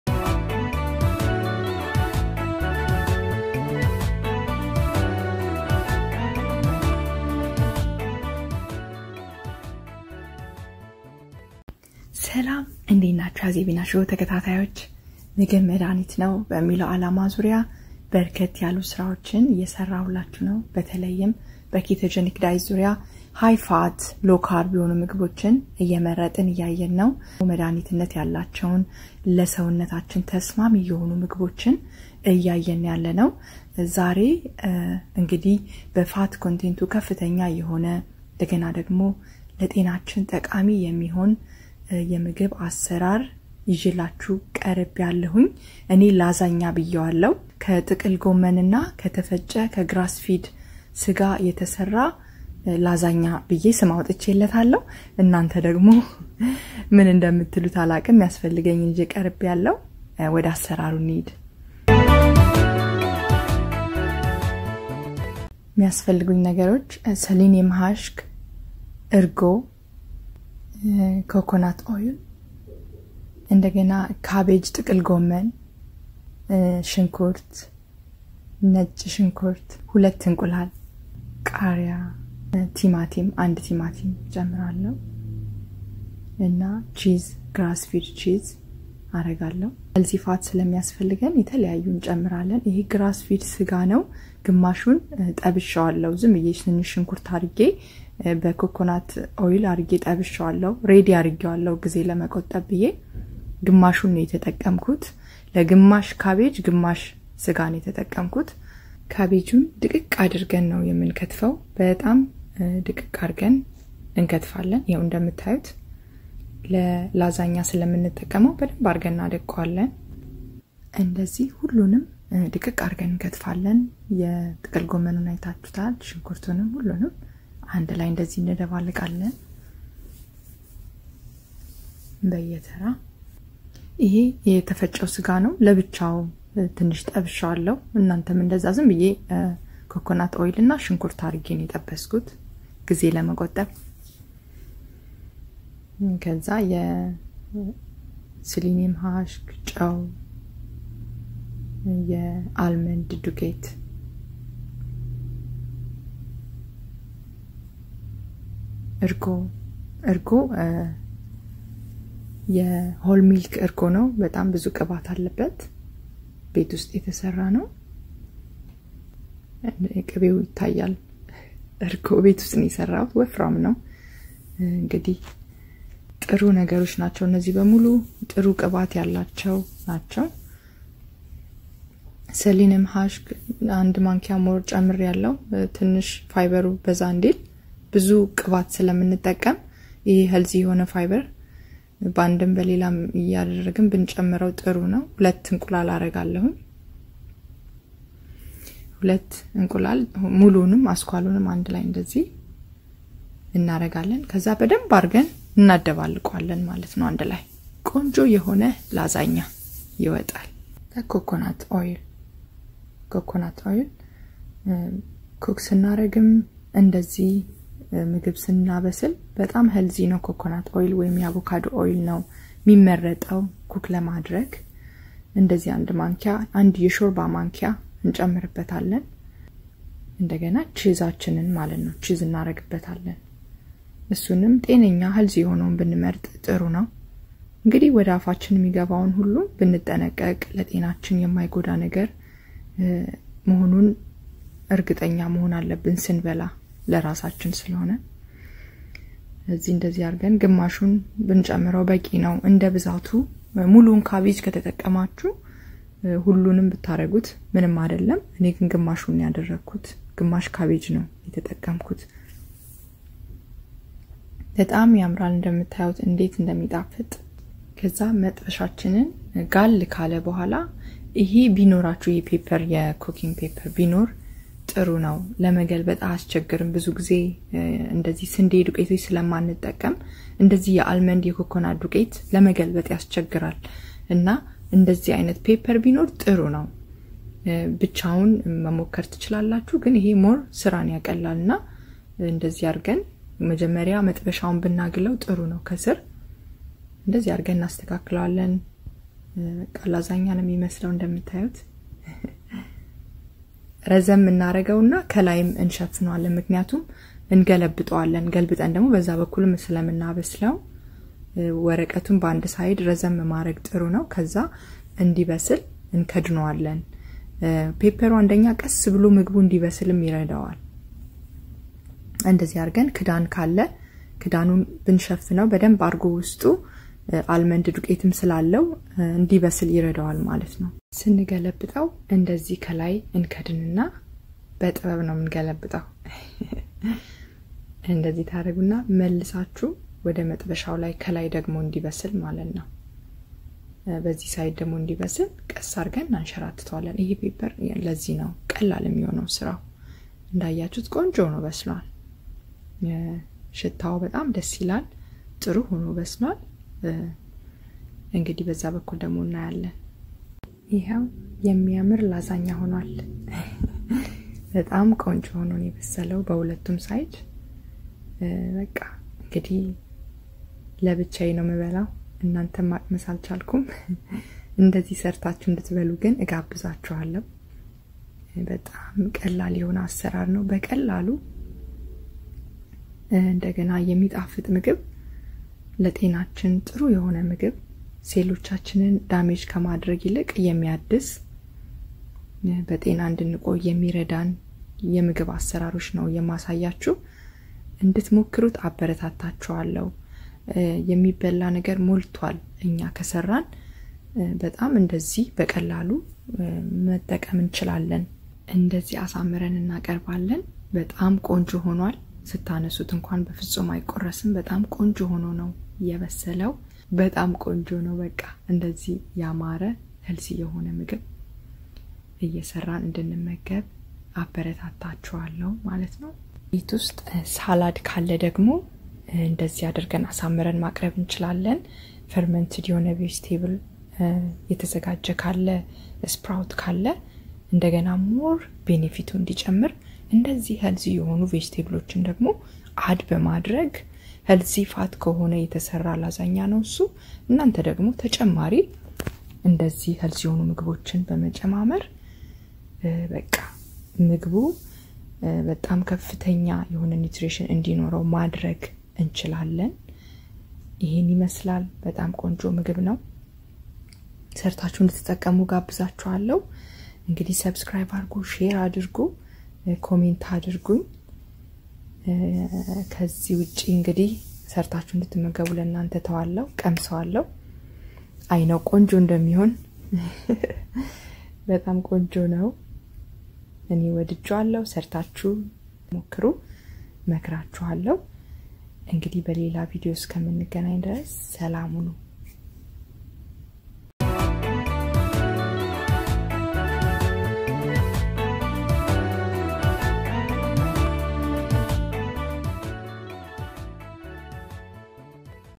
ETHIOPIA Ketogenic የላዛኛ አሰራር፣ ጤናችንን የማይጎዳ፣ እጅግ ቀላል፣ ከጥቅል ጎመን በመጥበሻ የተሰራ/ Cabbage Lasagna for Keto
Azebina Show
Ketogenic diet
Amharic
Ethiopia